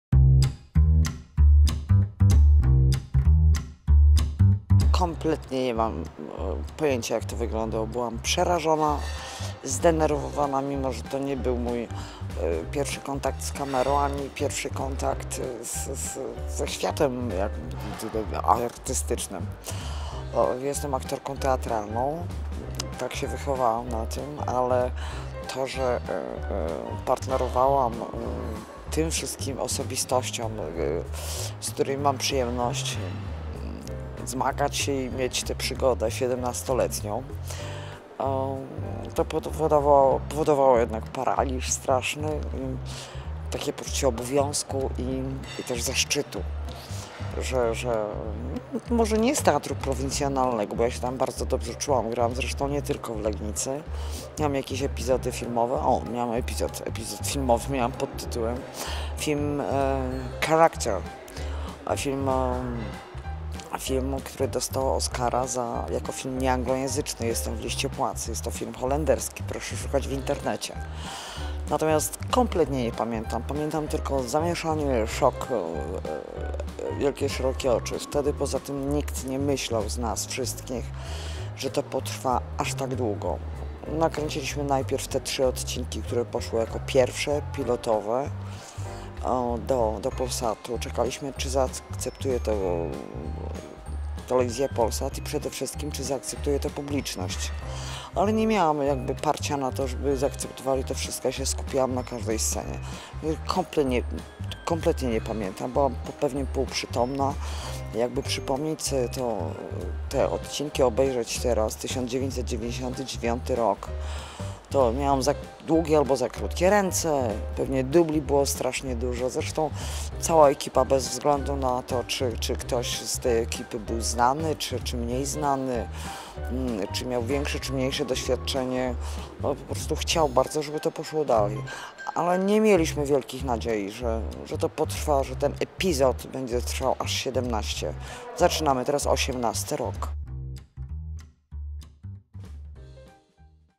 "Świat według Kiepskich": Marzena Sztuka wspomina pierwszy dzień!
Więcej kulis z planów realizowanych przez nas produkcji dostępnych na oficjalnej stronie i profilu producenta, firmy ATM Grupa: